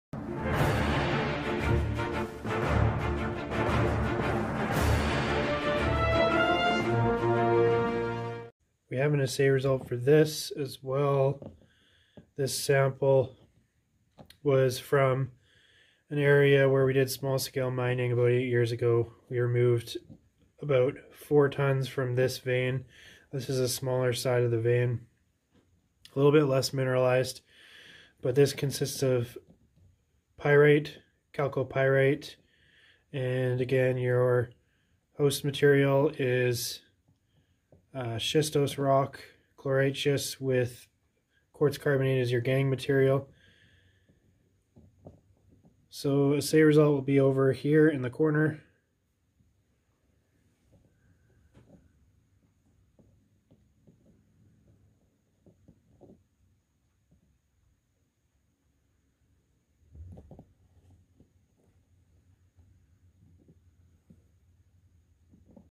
We Mined This Gold Deposit 8 Years Ago - Mineral Specimen Assay Result! (Ep. #10)
An assay and sample from a gold deposit we mined out 8 years ago. It doesn't look like much but the vein averaged 2oz/ton gold. With some silver and copper.